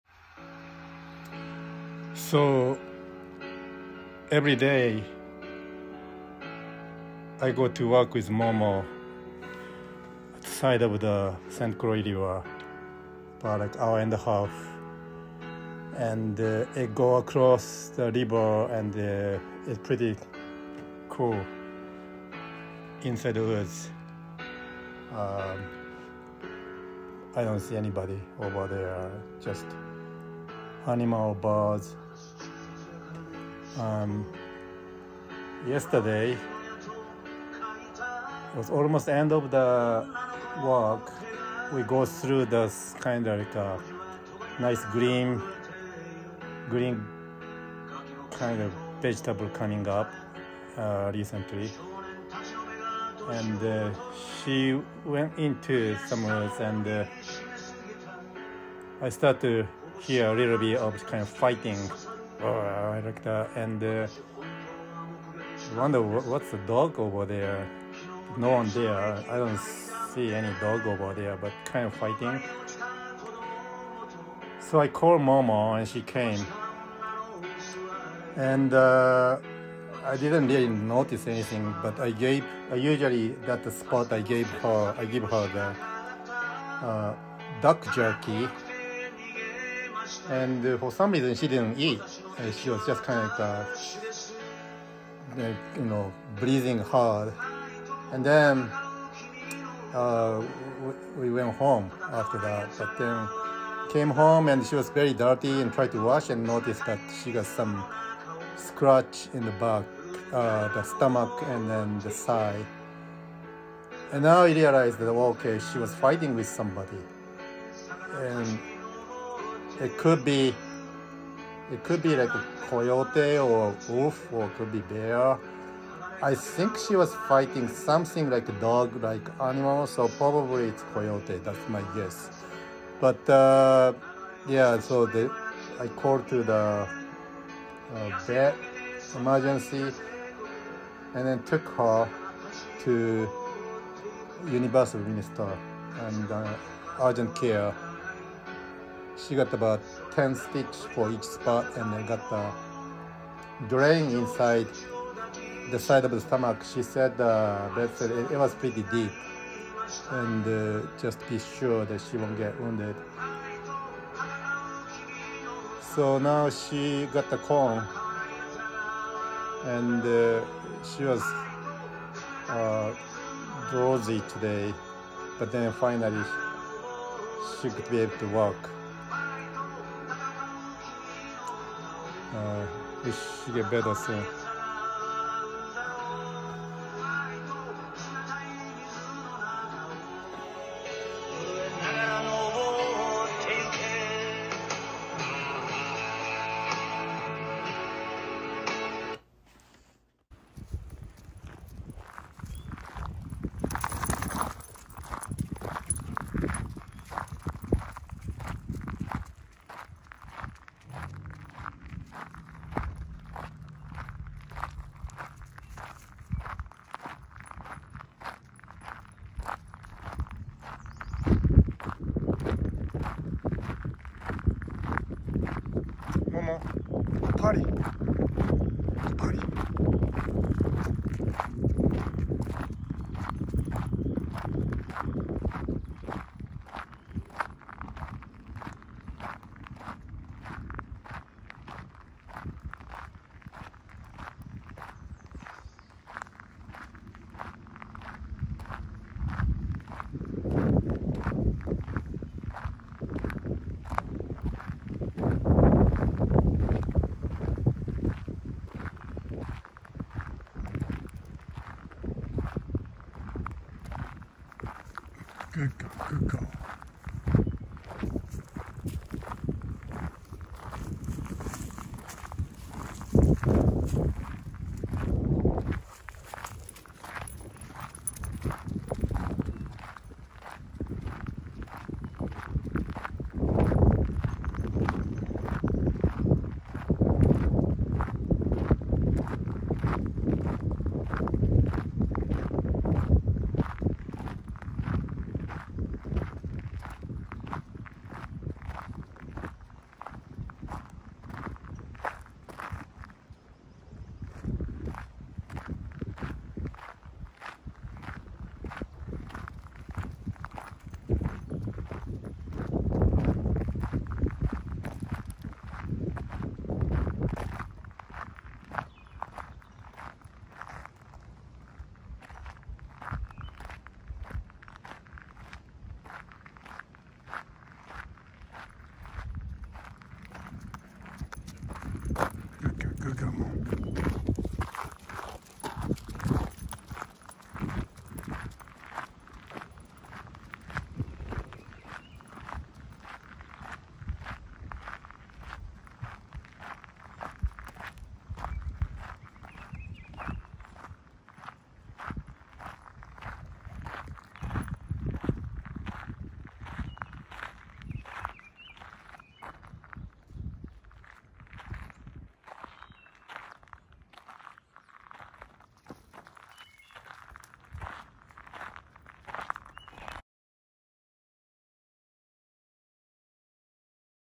Samejima - 20220514 Momo Fought
二千二十二年、五月十三日、満月の日に事件発生：コヨーテ、オオカミか・・・もも、戦う